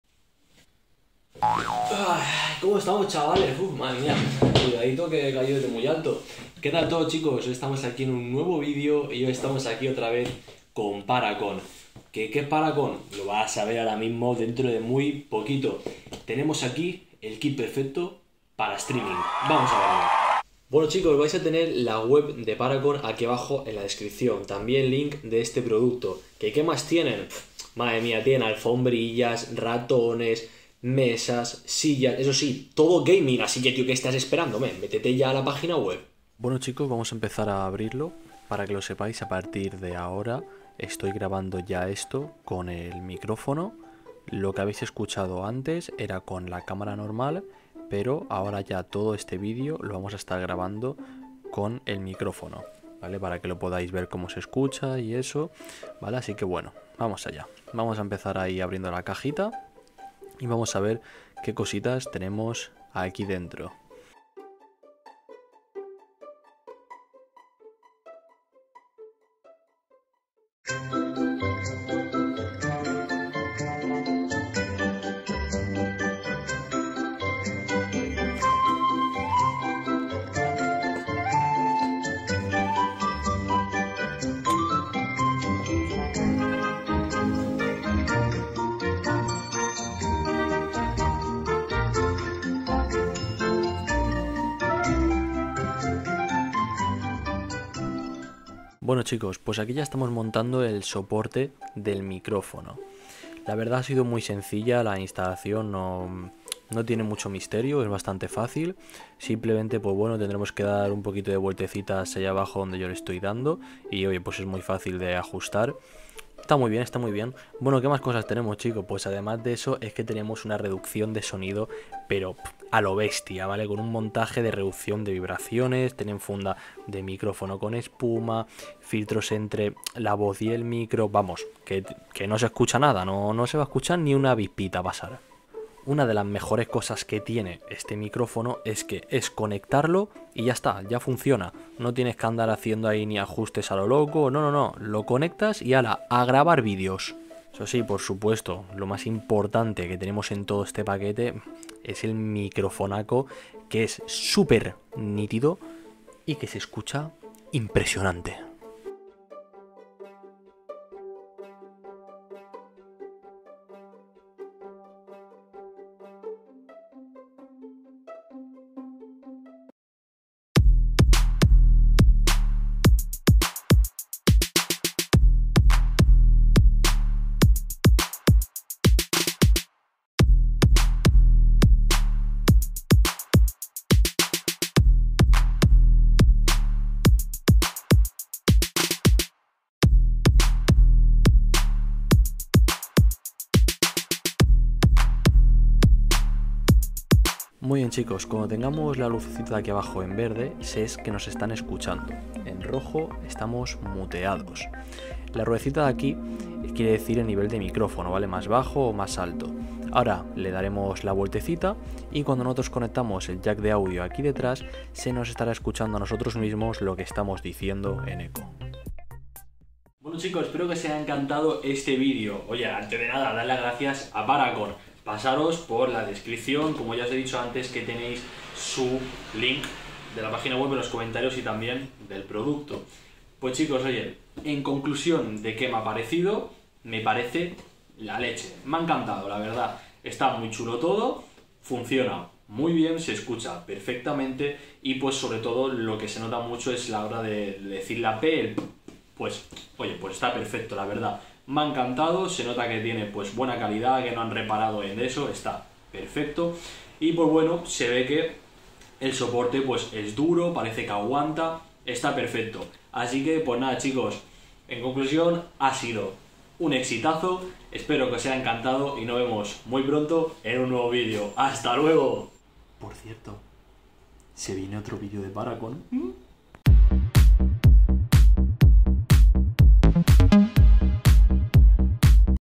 ¡EL MEJOR MICRÓFONO para STREAM!
¡MEJORA LA CALIDAD DE TUS VIDEOS CON EL KIT DE MICRÓFONO GAMING DEFINITIVO!  // PARACON REVIEW

¡Hola a todos! En este video les presentaré el kit de micrófono gaming que revolucionará la calidad de audio de tus videos. Este kit es perfecto para streamers, youtubers, podcasters y gamers que desean una experiencia de audio excepcional y profesional.

El kit incluye un micrófono condensador de diafragma grande que captura el sonido de forma clara y detallada, con una respuesta de frecuencia de alta calidad para que puedas grabar tu voz con un tono natural y sin interferencias. Además, cuenta con un soporte de brazo ajustable y una pantalla antiviento para eliminar los ruidos de fondo y el ruido de la respiración.

En el video podrás ver cómo funciona en diferentes situaciones de grabación y cómo se integra perfectamente con tus programas de edición de audio y video favoritos. También te hablaré de sus características especiales como la compatibilidad con USB y la tecnología de cancelación de ruido, que minimiza los sonidos ambientales no deseados.

No te pierdas este video si estás buscando una solución todo en uno para tus necesidades de audio.